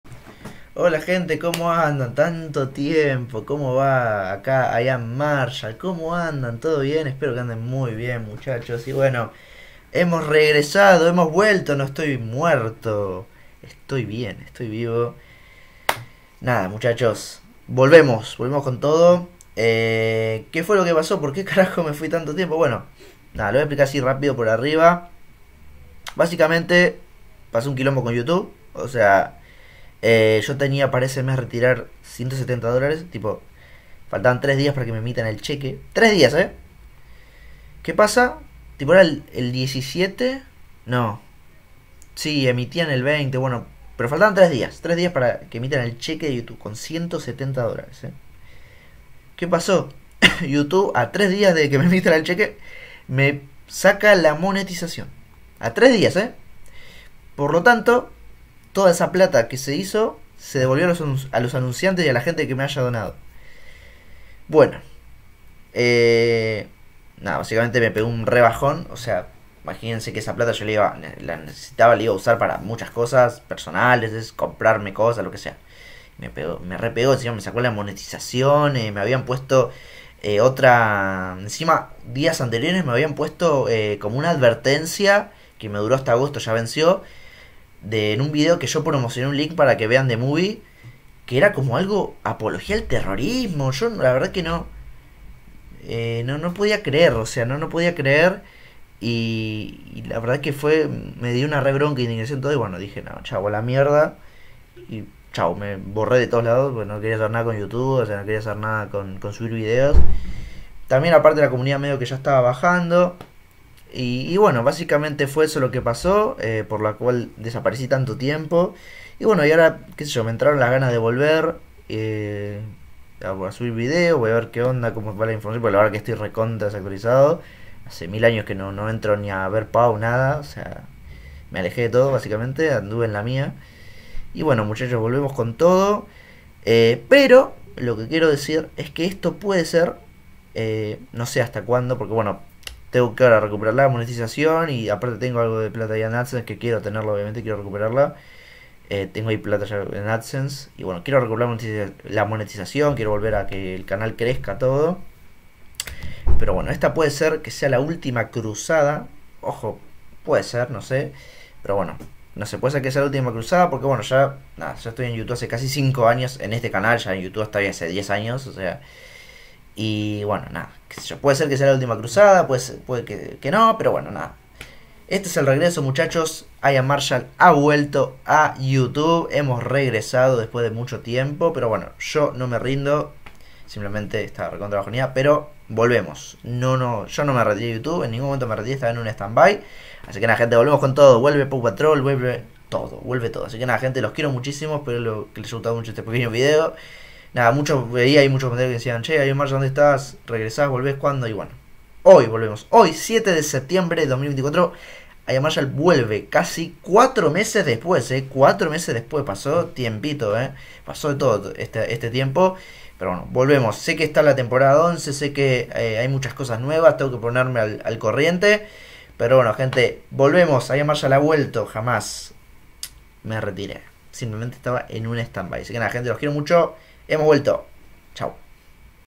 HEMOS VUELTO. PORQUE ME FUI TANTO TIEMPO???
IAMMarshall HA REGRESADO. PORQUE ME FUI TANTO TIEMPO DE YT? LAS RAZONES LAS EXPLICO EN EL VIDEO!